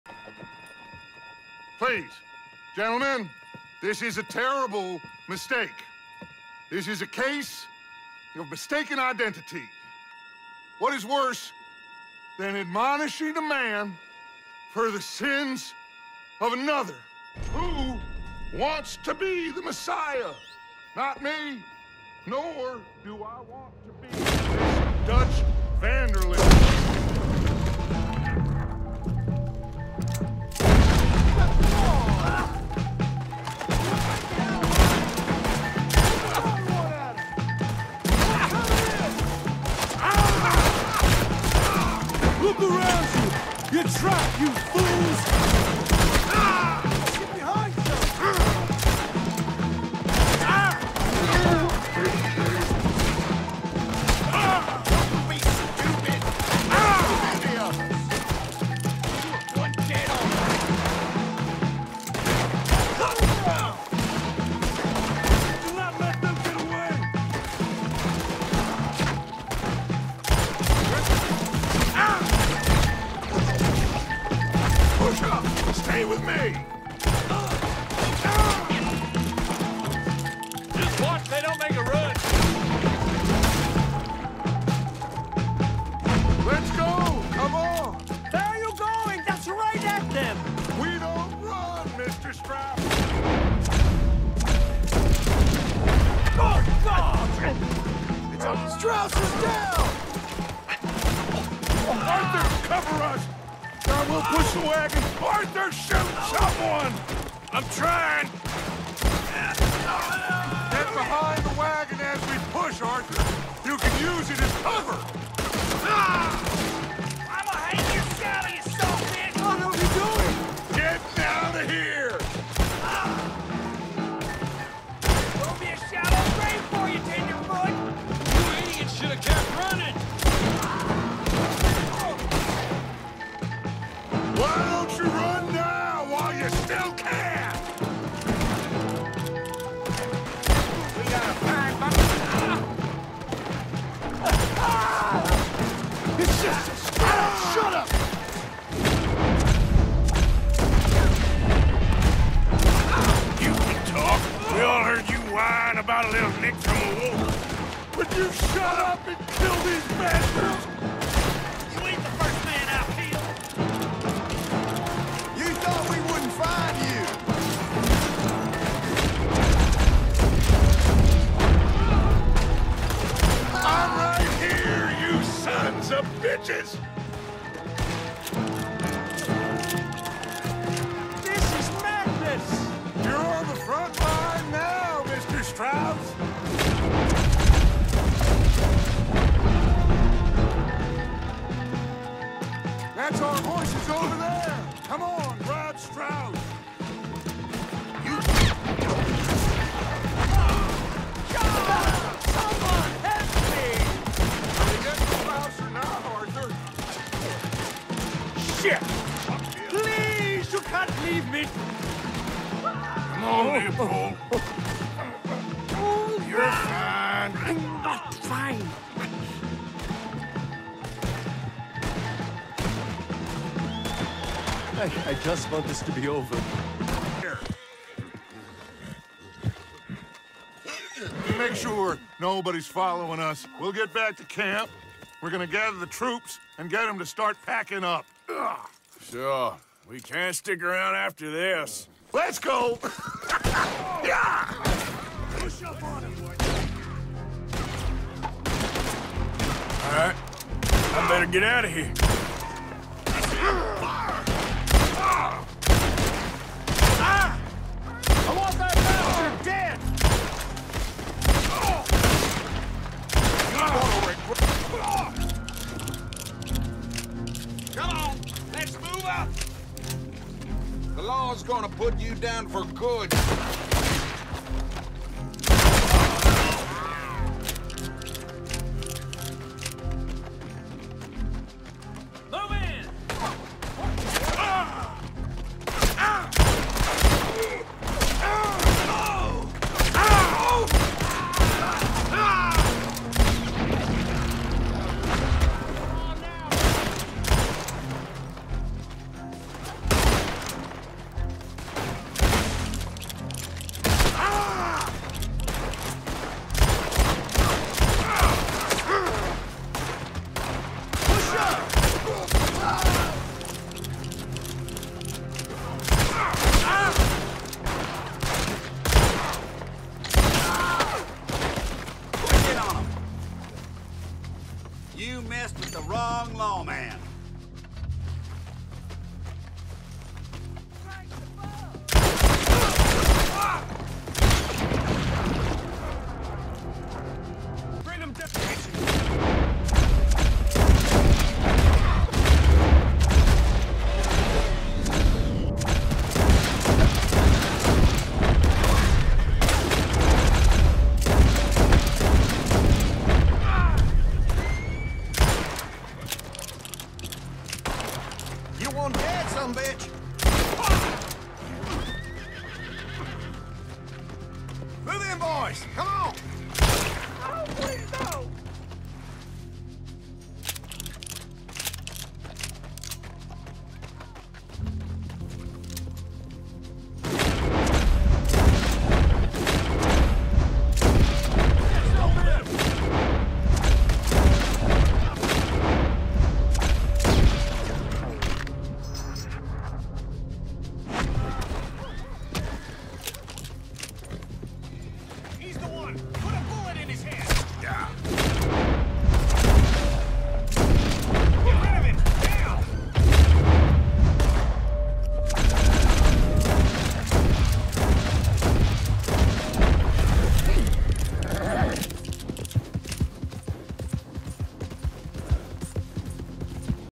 This is peak gaming
The combination of the excellent & tense plot, flawless acting & banter, well thought out mission design and most of all, brilliant action gameplay, with wonderfully - and at times, disturbing - reactive animations to all of the actions, turns all of this into an almost perfect gaming experience, even though the movement in first person mode, is not as smooth as it is in dedicated FPS games.